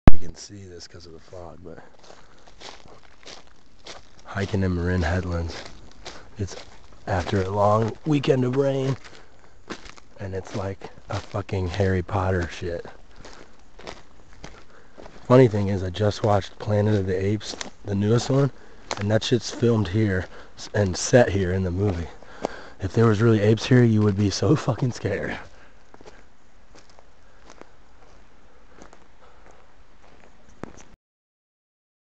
Hiking in Muir Woods (Where Planet of The Apes was filmed)
Foggy awesomeness...kinda spooky right? This is where they filmed the newest planet of the apes movies.